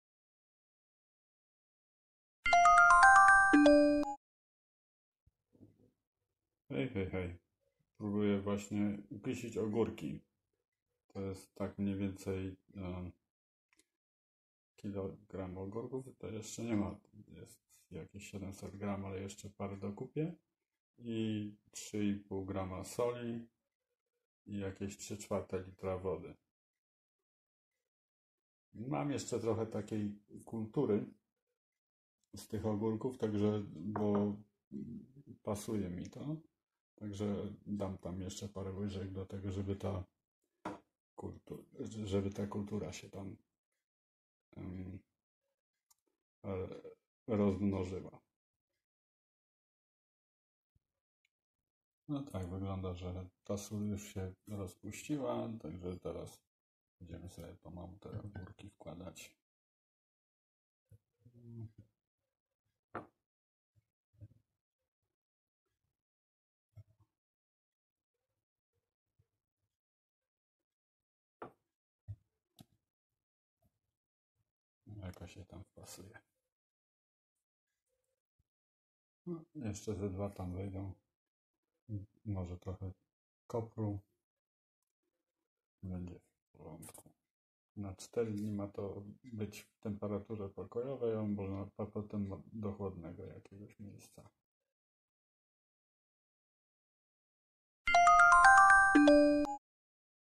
20190505 Kiszenie Ogórków w Słoikach
Tao Mola Książkowego, kiszenie ogórków wedle przepisu z Kuchni Polskiej.